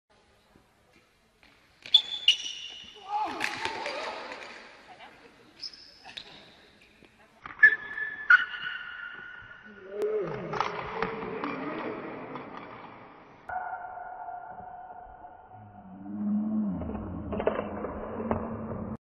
Badminton Player Fell Down
This badminton player shows up everyone else on the court by playing both offense and defense, at the same time